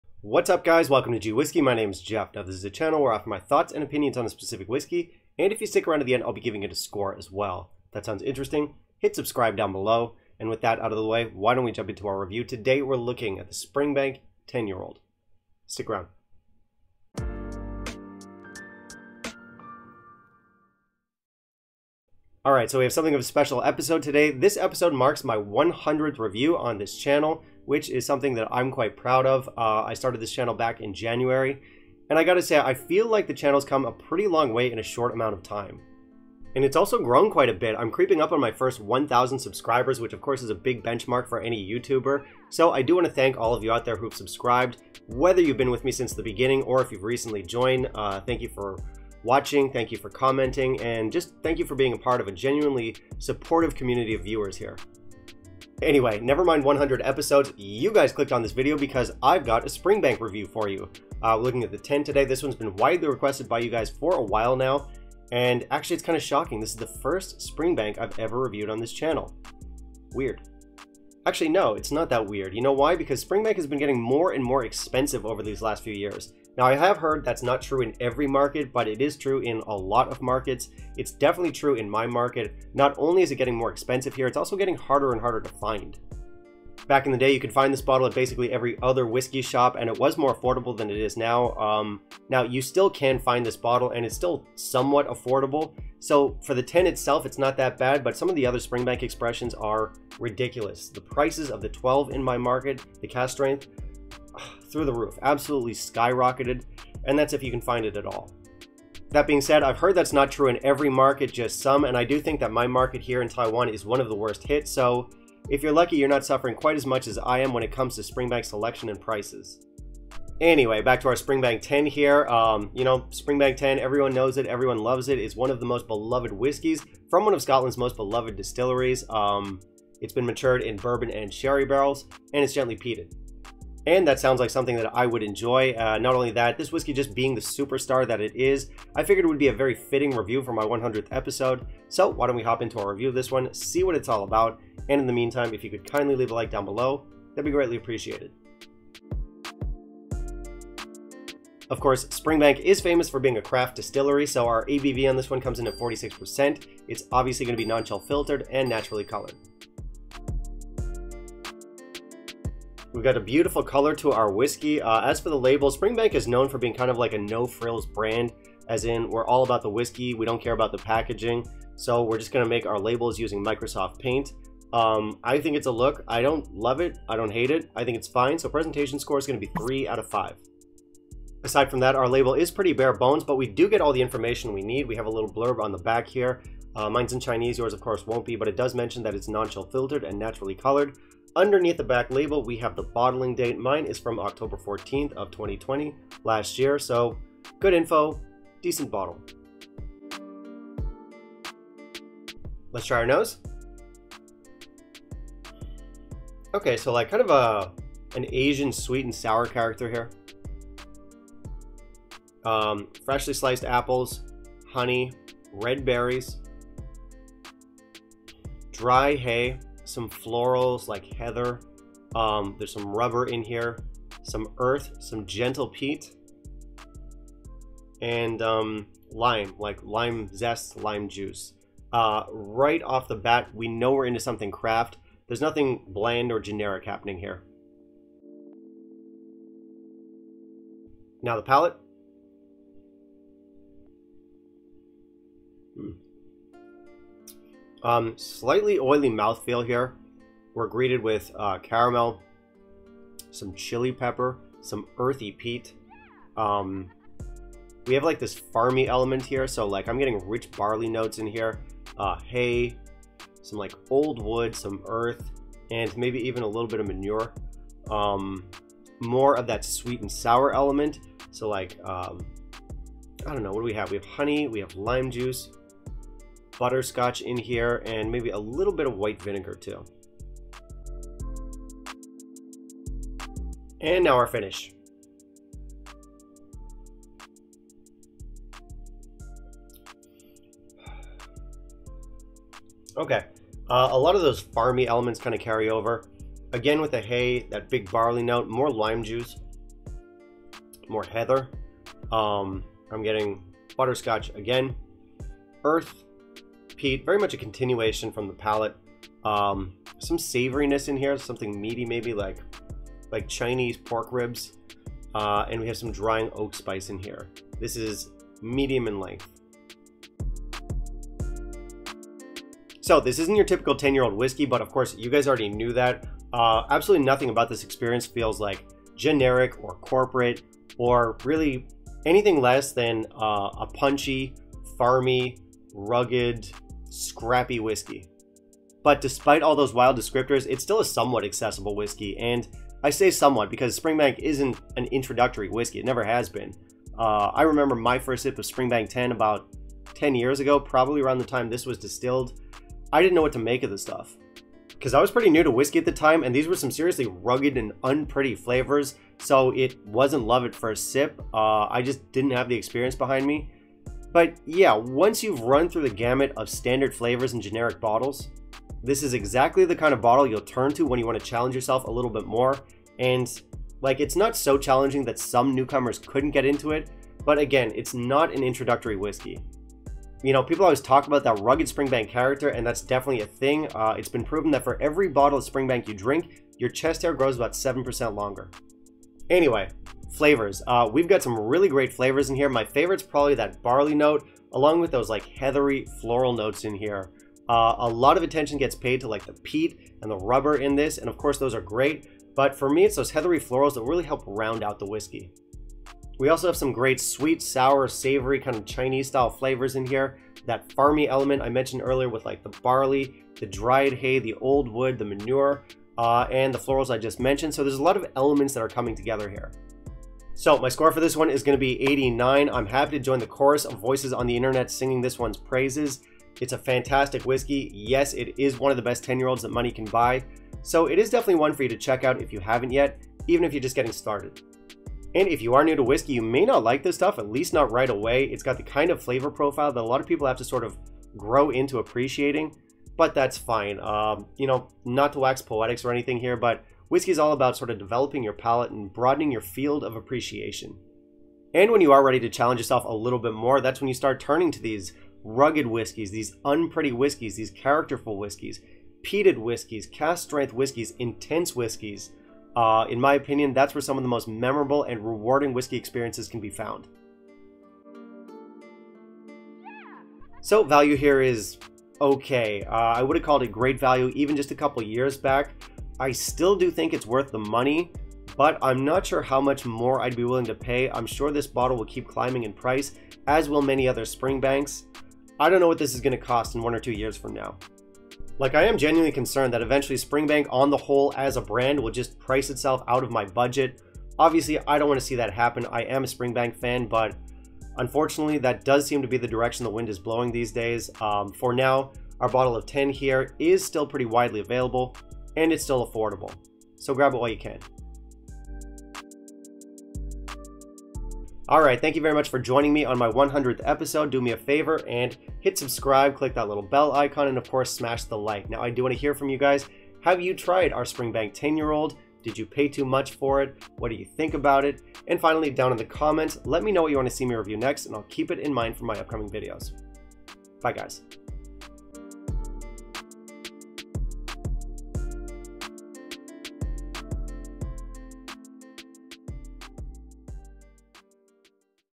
Springbank 10 REVIEW: The BEST 10 YEAR OLD?
It's my 100th episode today. I figured I'd celebrate with one of the most beloved entry level single whiskies from Campbeltown. This is the Springbank10. I'll taste it and give you my thoughts. Cheers!

Scores:
0-75 - Crap
75-79 - For cocktails & people you don't like
80-82 - Not bad
83-85 - Getting good here
86-89 - Delicious
90-94 - Awesome
95-100 - Cowabunga Dudes